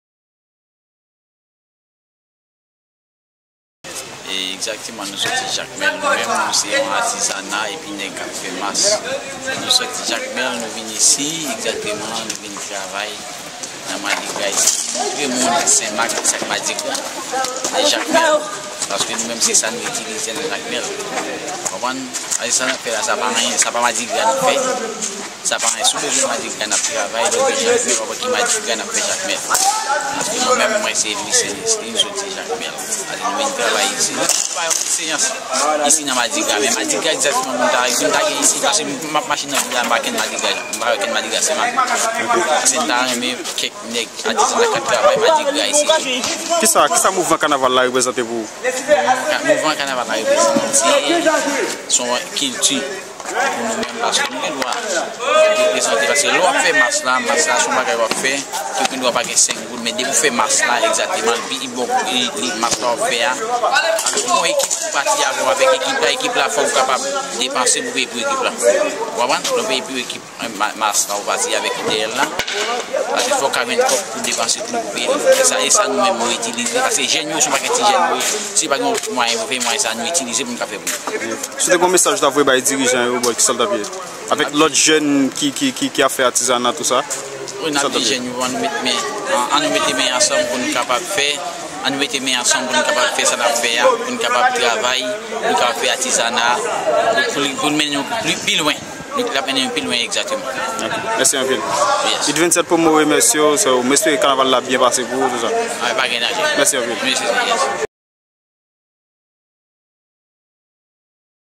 Nesly soti Jakmel vin patisipe nan kanaval Senmak
Atizan an di li vin montre moun senmak sak rele madigra